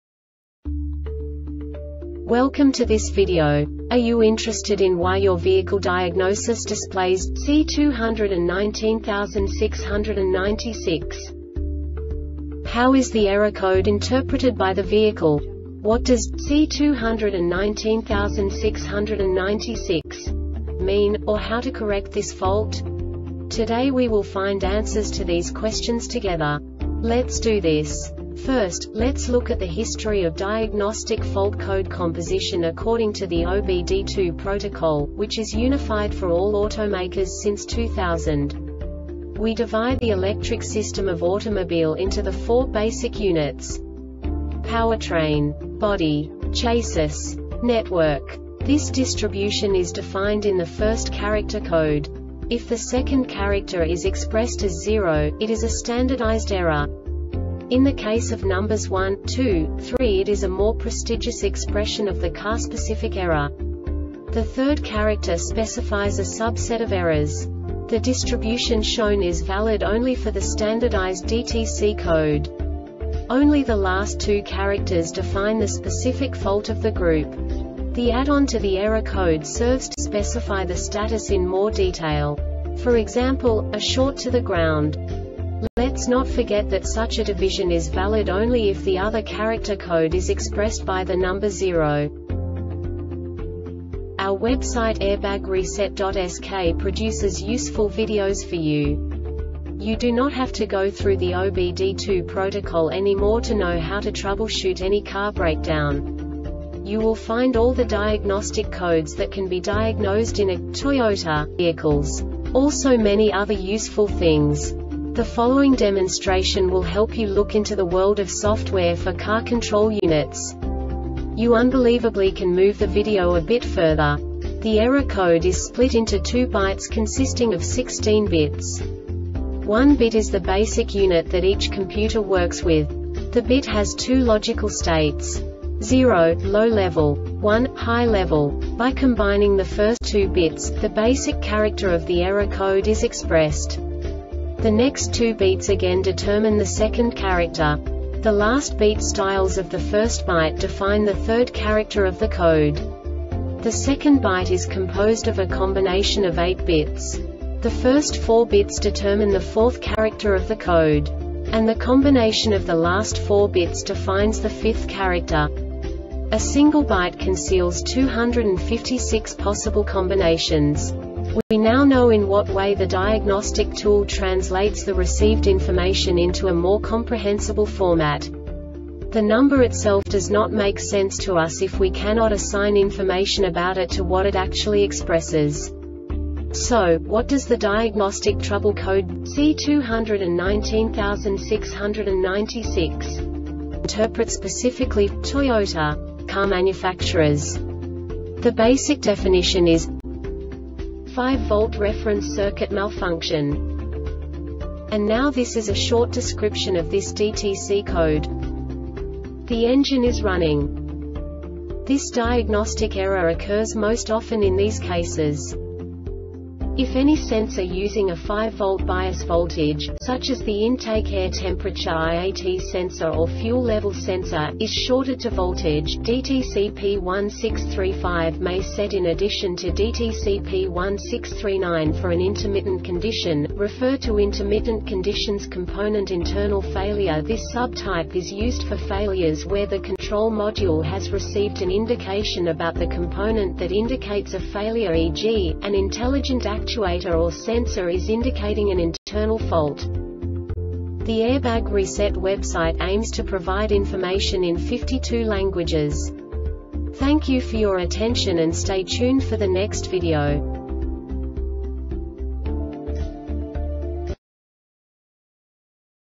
DTC Toyota C2196-96 Short Explanation
Contents:
0:21 Basic DTC analysis according to OBD2 protocol standard.
1:48 Insight into programming
2:58 A diagnostically understandable description of the Diagnostic Trouble Code
3:05 Basic error definition

Make:
Toyota
Code:
C2196 96
Definition:
5-Volt Reference Circuit Malfunction
Description:
The engine is running.
Cause:
 If any sensor using a 5-volt bias voltage,such as the intake air temperature (IAT) sensor or fuel level sensor,is shorted to voltage,DTC P1635 may set in addition to DTC P1639.For an intermittent condition,refer to Intermittent Conditions .